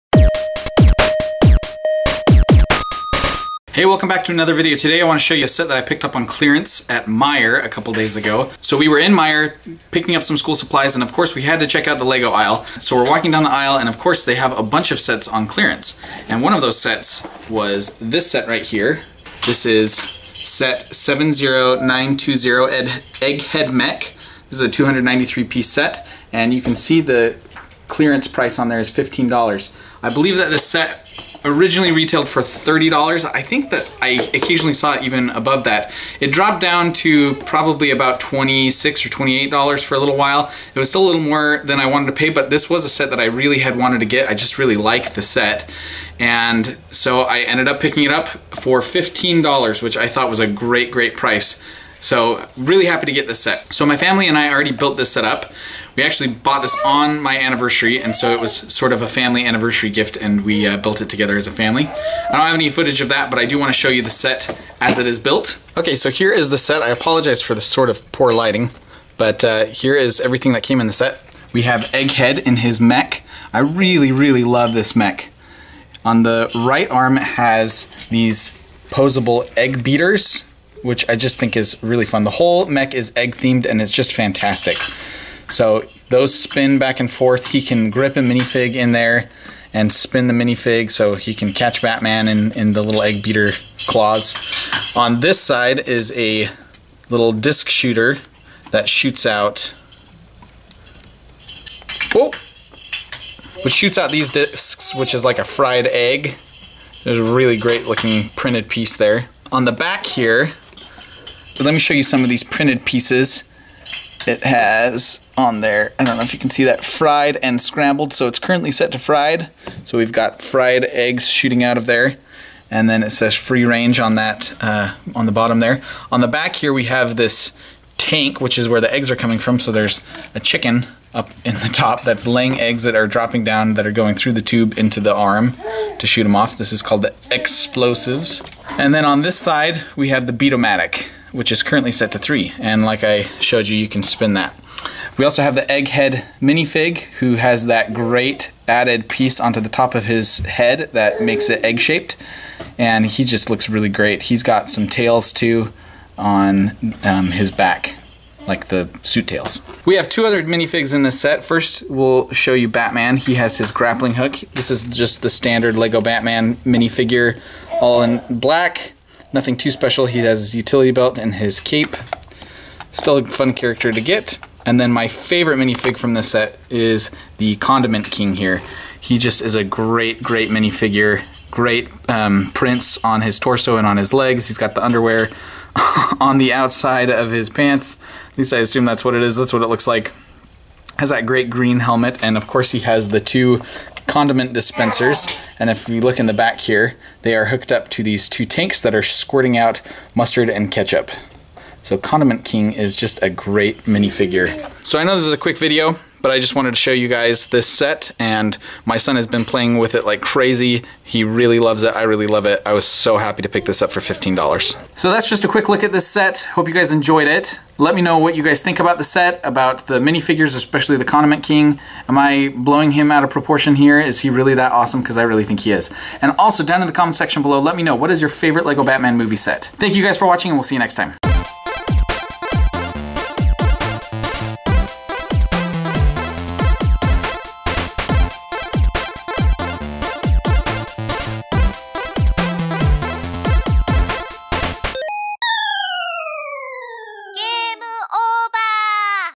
Clearance LEGO Batman set 70920 Egghead Mech Food Fight
I found a LEGO set on clearance at Meijer!